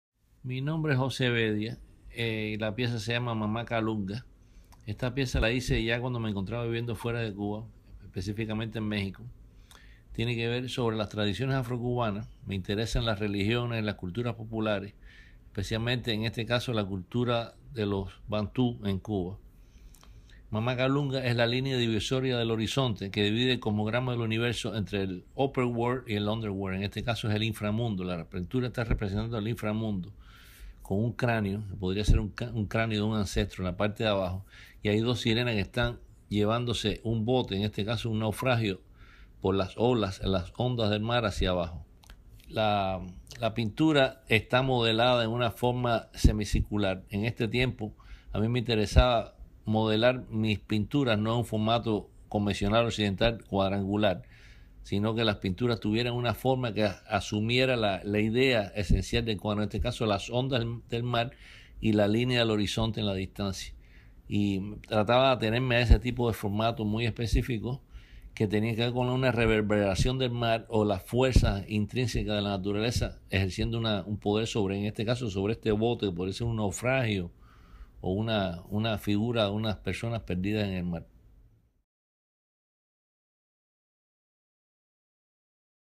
José Bedia talks about his work
José Bedia’s interest in AfroCaribbean cultures is both personal and anthropological. He is a scholar and practitioner of the Afro-Cuban religion Palo Monte, whose traditions are rooted in religions that arrived in Cuba with Central African slaves. They believed that the voyage across the ocean brought them to the land of the dead. Mamá Kalunga, whose name is written on the skull emerging from the bottom of a frayed semi-circular canvas, is the Palo Monte fertility spirit.

In this video Bedia speaks about his work, Mamá Kalunga (1992), which is included in the exhibition "PortadoresDeSentido: Contemporary Art from the Colección Patricia Phelps de Cisneros" and is on view at the Museo Amparo in Puebla, Mexico through July 22, 2019. Find more audio capsules featuring artists talking about their works

José Bedia (Cuba, 1959), Mamá Kalunga, 1992. Acrylic and wood on canvas. 170 × 300 × 10.2 cm. Colección Patricia Phelps de Cisneros.